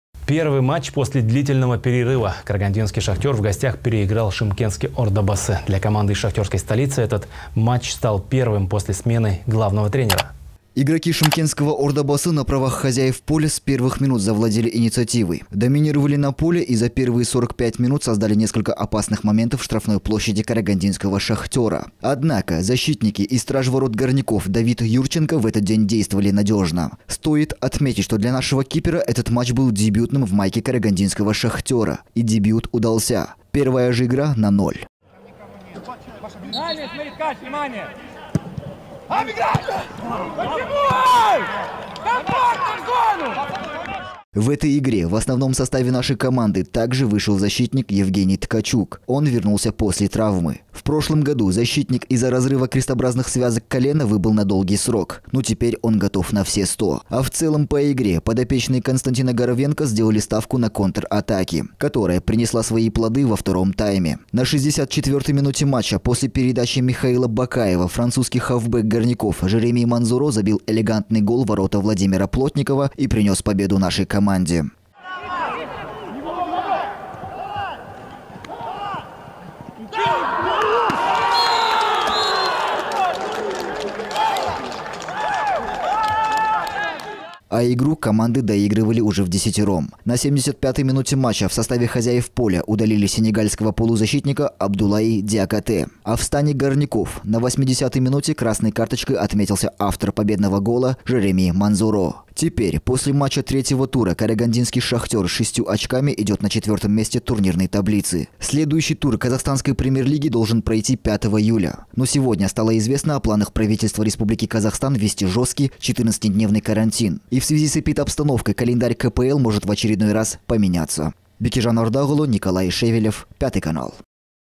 Первый матч после длительного перерыва
Карагандинский "Шахтер" в гостях переиграл шымкентский "Ордабасы".